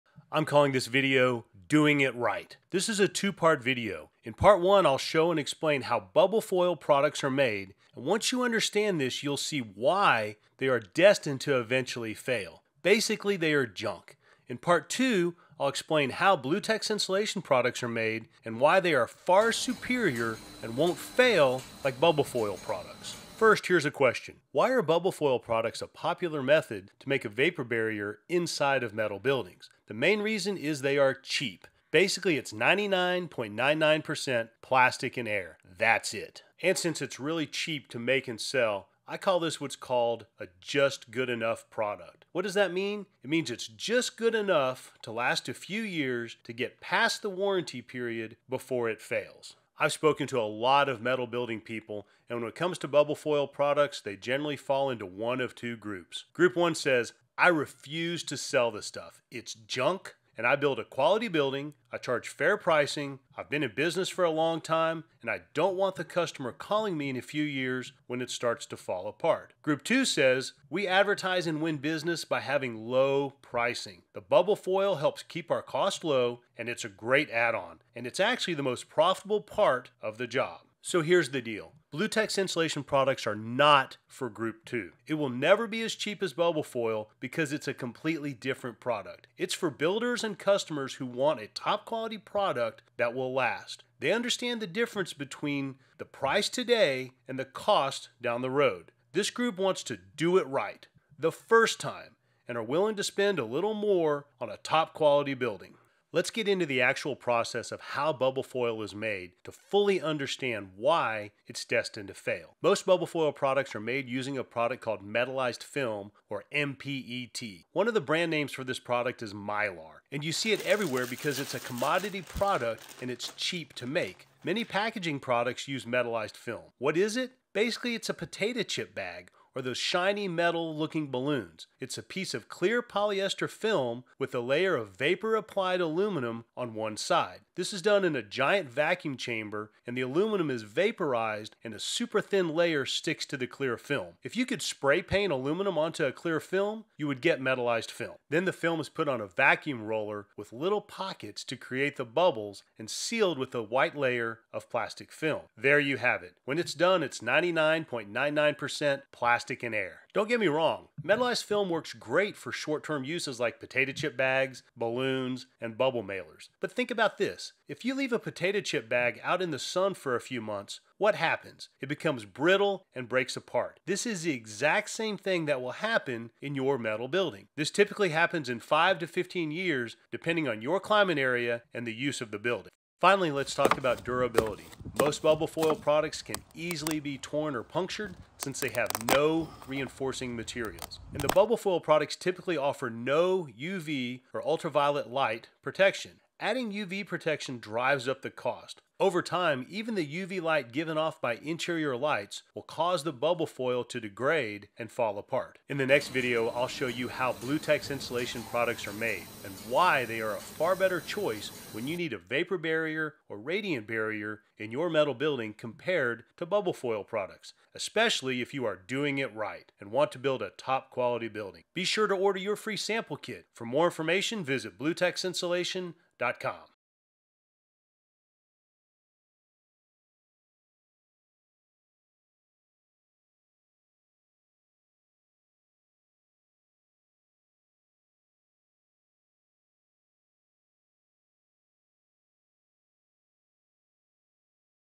Bubble Foil Insulation Deterioration - It's Actually ONLY A Potato Chip Bag!
This is Part 1 Of a 2-part video. This video explains how bubble foil insulation is made from Mylar or metalized film.  This is basically a potato chip bag! And, in 5-15 years in most metal buildings, the radiant heat and the UV from the lights will cause it to break down and disintegrate. So it's just "good enough" to last a few years before it fails. is a superior product that uses pure aluminum rolls (instead of Mylar) and high density EPE or EXP foam instead of bubbles.  This creates a product that will last, and won't dry out and become brittle.